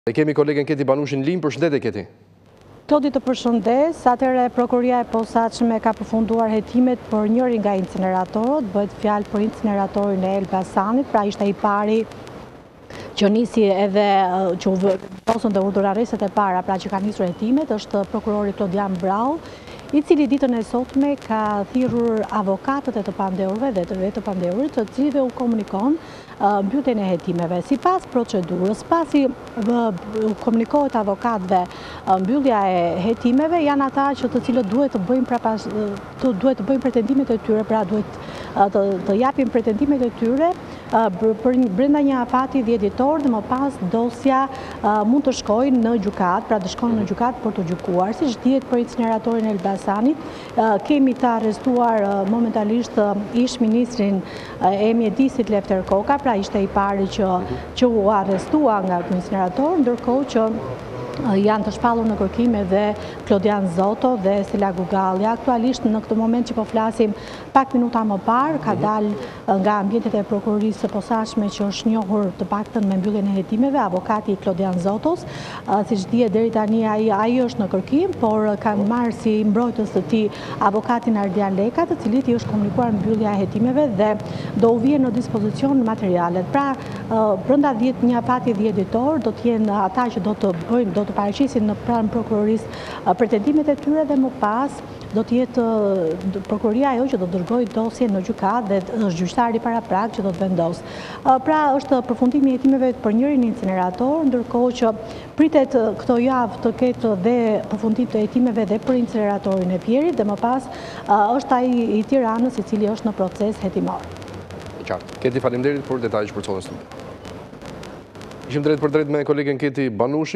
SPAK përfundon hetimet për incineratorin e Elbasanit, u komunikohet akuza Kokës, Gugalles dhe Zotos
Përfundojnë hetimet për incineratorin e Elbasanit.

Prokurori Klodian Braho u ka komunikuar avokatëve të të pandehurve akuzat.

"Sot avokatët e të pandehurve u njohën me përfundimin e hetimeve. Sipas procedurës pasi u komunikohet mbyllja e hetimeve avokatët duhet të dorëzojnë pretendimet e tyre brenda afatit 10 ditor dhe më pas dosja shkon në gjykatë dhe është gjyqtari paraprak që do të vendosë" informon gazetarja e Ora News, Keti Banushi. Për incineratorin e Elbasanit është i arrestuar ish Ministr Lefter Koka, ndërsa në kërkim janë Klodian Zoto dhe Stela Gugallja.

Të pandehur pritet të merren dhe ish zyrtarë të ministrisë së Mjedisit, informon Ora News.

Avokati i Klodian Zotos, Ardian Leka, si dhe avokatët e Lefter Kokës dhe Stela Gugalles u njohën sot me përfundimin e hetimeve

Ish-ministri i Mjedisit Lefter Koka u arrestua më 14 dhjetor 2021 me urdhër të Prokurorisë së Posaçme Kundër Korrupsionit dhe Krimit të Organizuar, në kuadër të hetimeve për koncesionin kontrovers të inceneratorit të Elbasanit.

Stela Gugallja, e cila është pronarja e kompanisë Albtek Energy si dhe Klodian Zoto akuzohen për veprat penale “Korrupsioni aktiv i funksionarëve të lartë shtetërorë ose i të zgjedhurve vendorë”, kryer në bashkëpunim, si dhe “Pastrimi i produkteve të veprës penale apo i veprimtarisë kriminale”.

Me vendimin e 14 dhjetorit 2021 u vendos dhe sekuestroja e incineratorit.  SPAK shpjegoi atëherë se Lefter Koka, në cilësinë e ministrit të Mjedisit ka shkelur procedurat për dhënien e licencës dhe miratimin e vijimit të ndërtimit të objektit, ndërsa Klodjan Zoto dhe Stela Gugallja akuzohen se kanë kryer transaksione të rreme me paratë e marra nga buxheti i shtetit për llogari të kontratës koncesionare. Po ashtu ata i kanë dhënë ryshfet ish-Ministrit në mënyrë që ky i fundit të lejonte ndërtimin e inceneratorit.

Pritet që brenda javës të mbyllen dhe hetimet për incineratorin e Fierit.